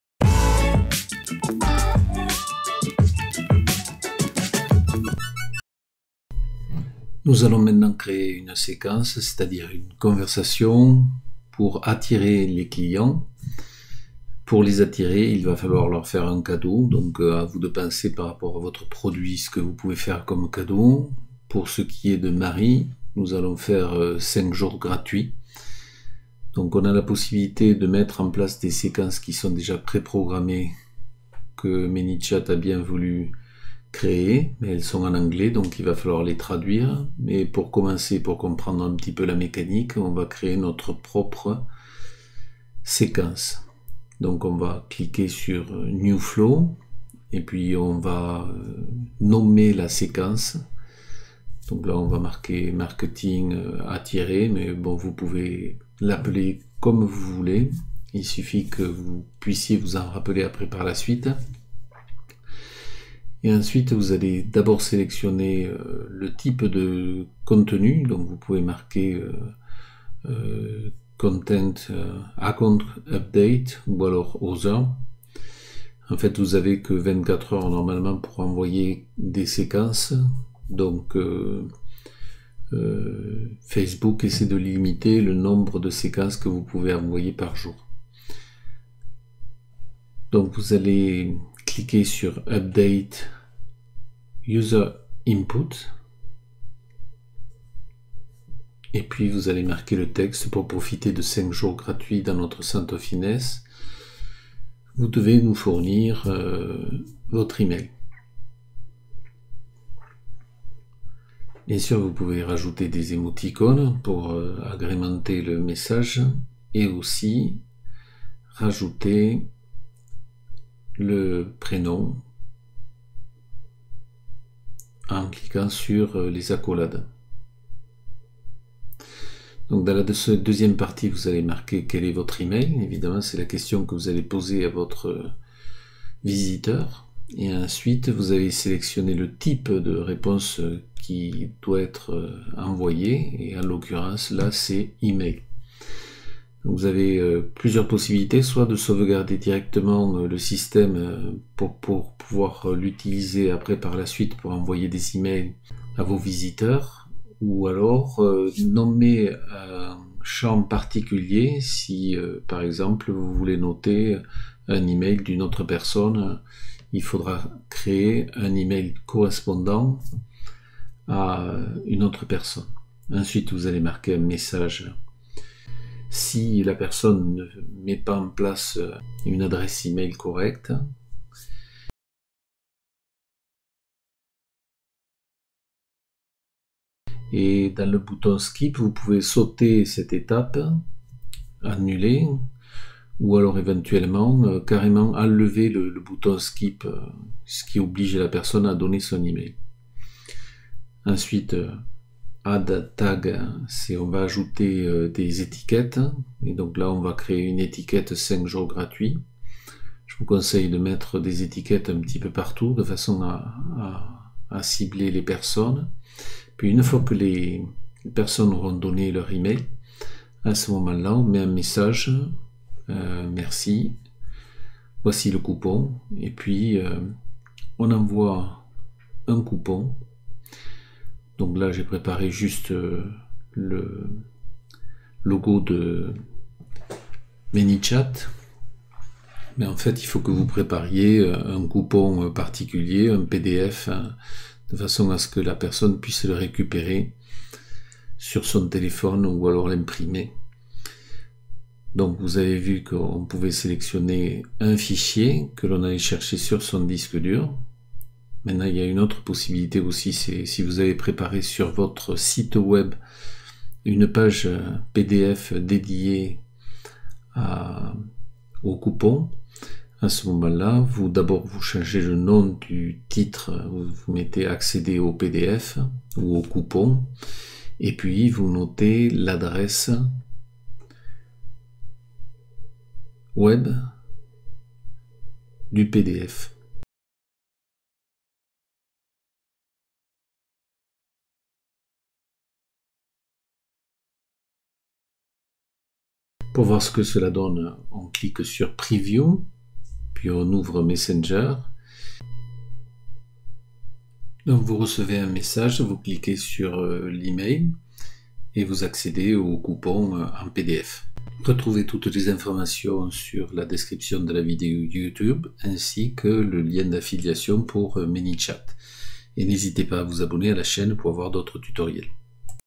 Comment attirer des visiteurs avec Manychat
Tutoriel
Pour attirer des visiteurs, vous devez leurs faire un cadeau. Il peut s'agir, d'un essai gratuit, d'un livre blanc, d'un service gratuit. Puis vous en profitez pour récupérer leur email.